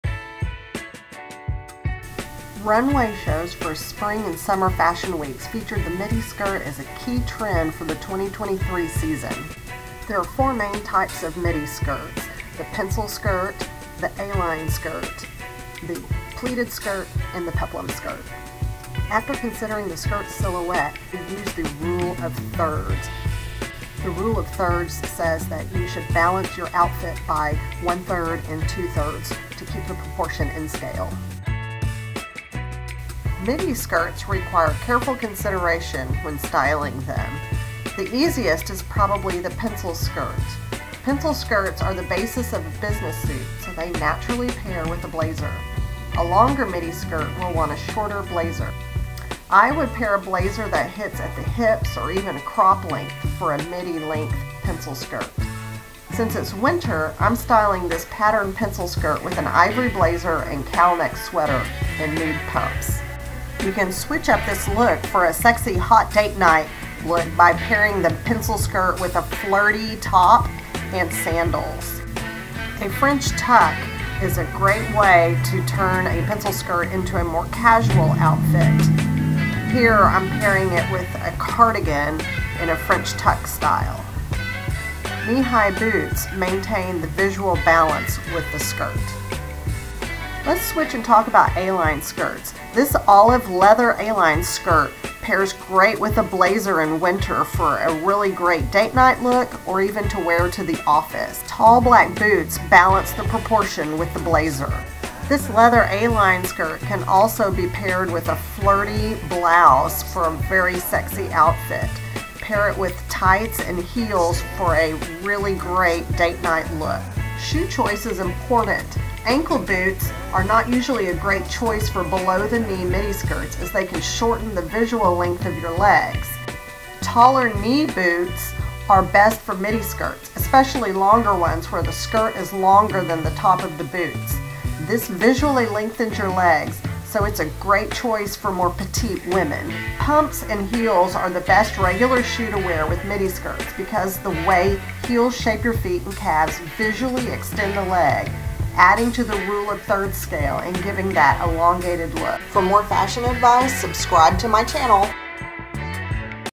How to style midi skirts for winter 2023
How to style midi skirts for winter 2023. Midi skirts are big this winter. This video talks you through how to style them, including how to choose which shoes to wear.